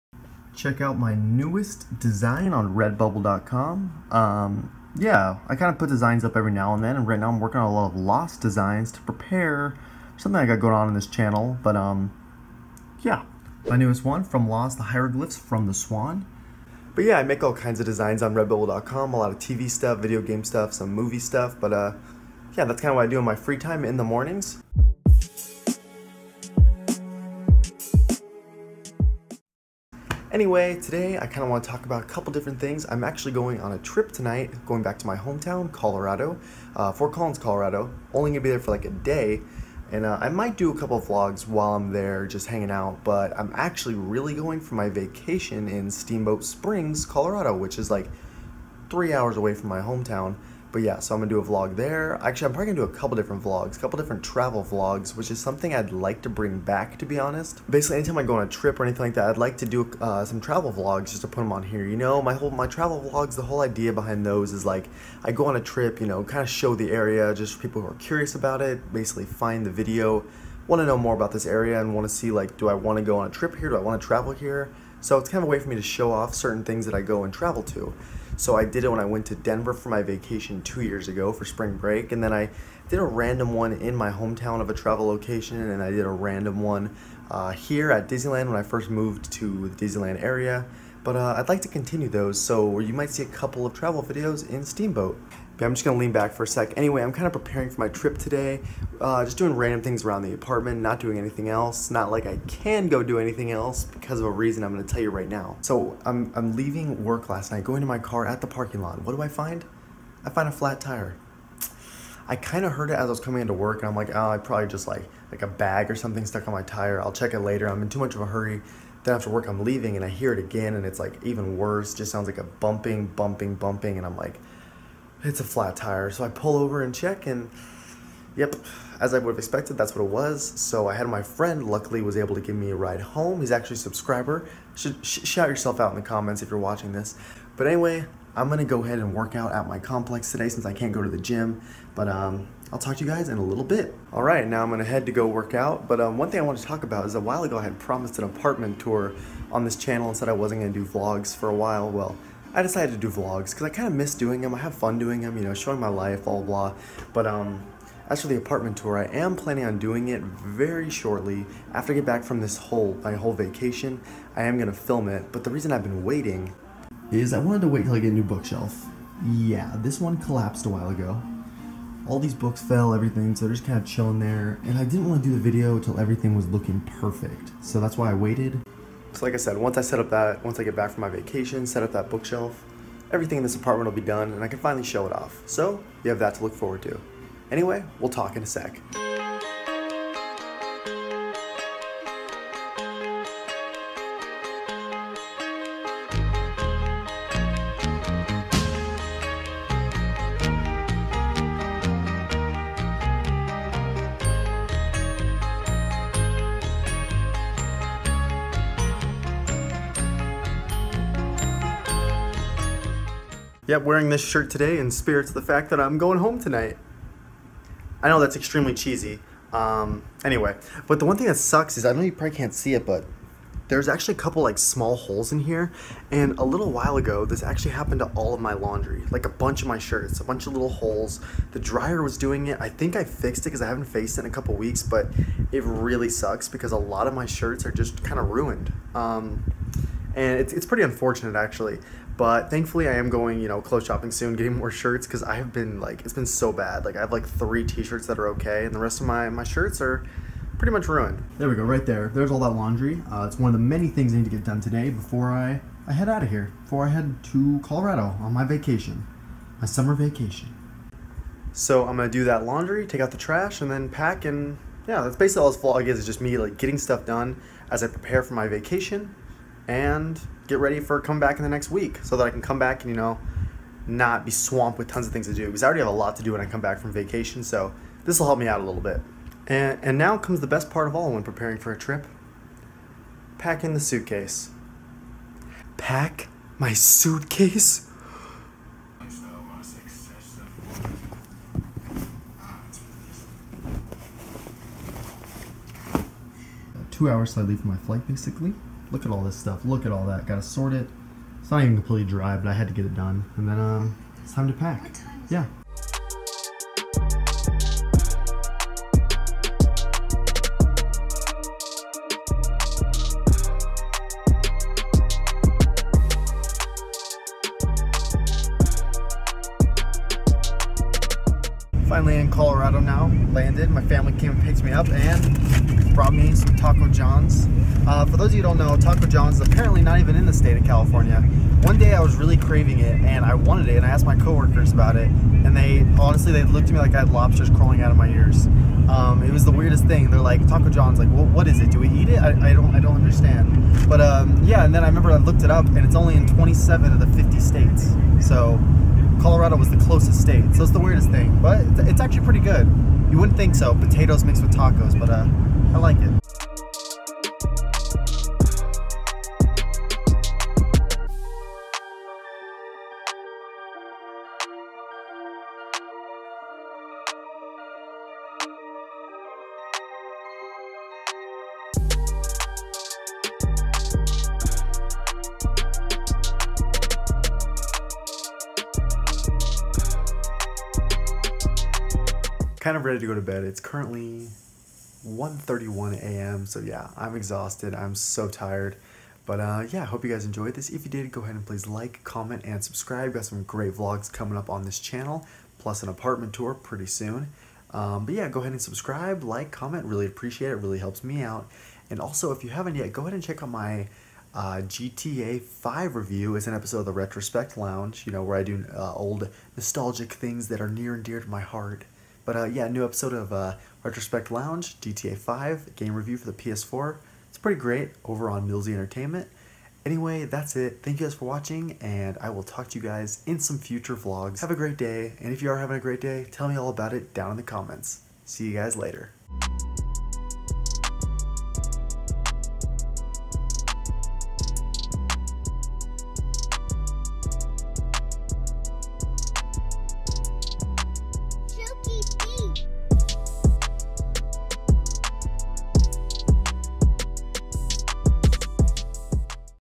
PREPPING for My End of SUMMER Vacation!
*VIDEO HIGHLIGHTS*
-My RedBubble Store
-Hitting the Gym
-Arriving in Colorado
-Relaxing for the Evening